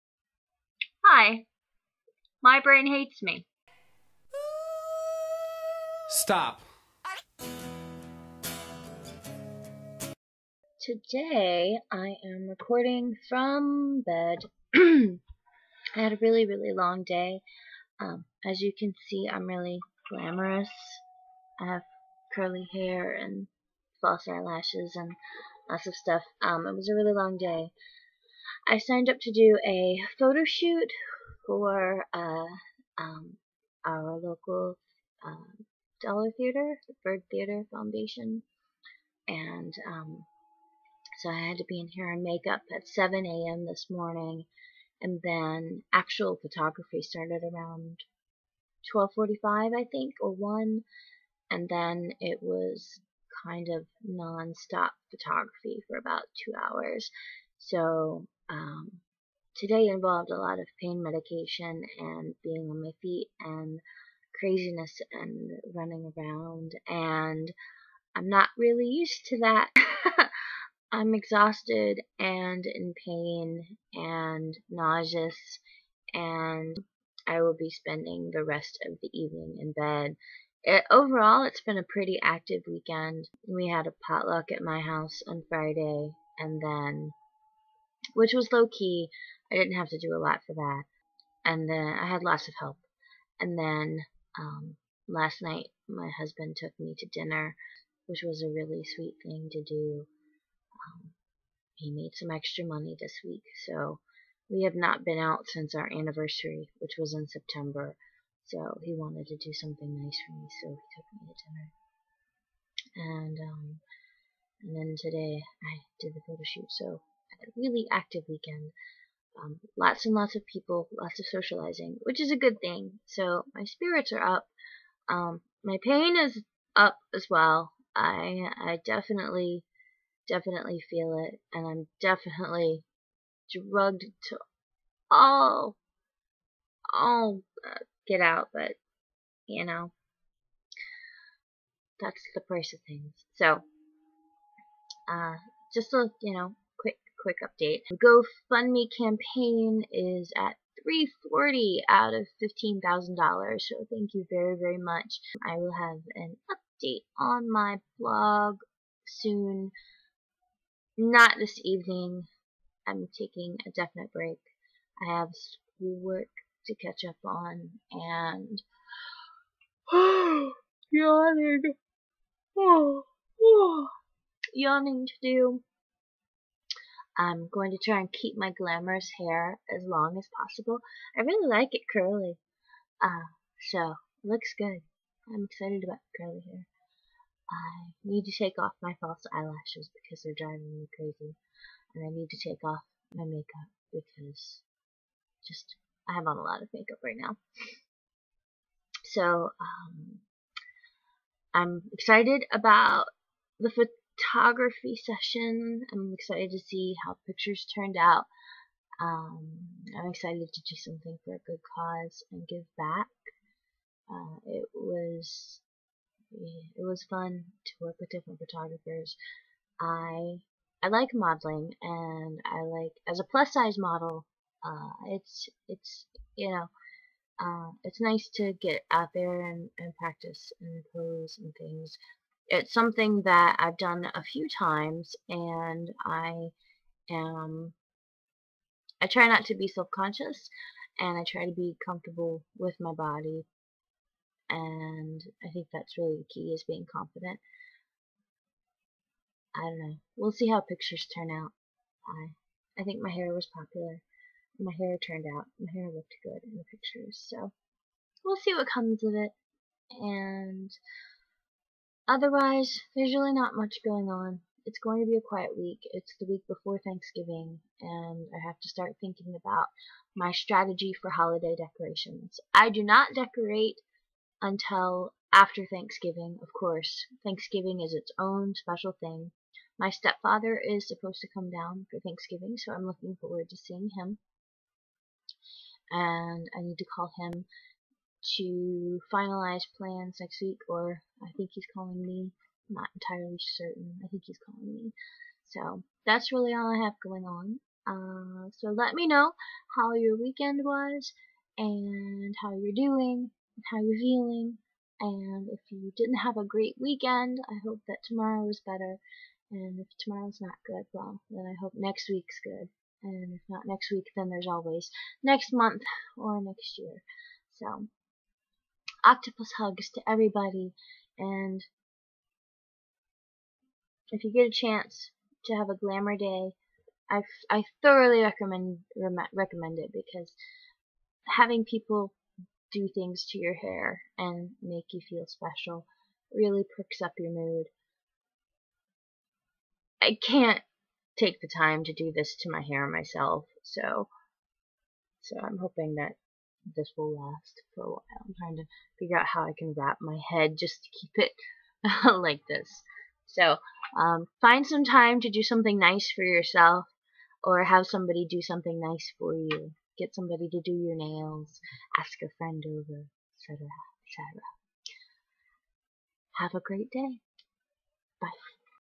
Pink Polka Dot Sherpa Robe Diaries: Glamourpus
All dolled up and an activity hangover to boot! What's an activity hangover? It's this thing where someone with chronic pain tries to do stuff like a normal person and then it takes a week to recover.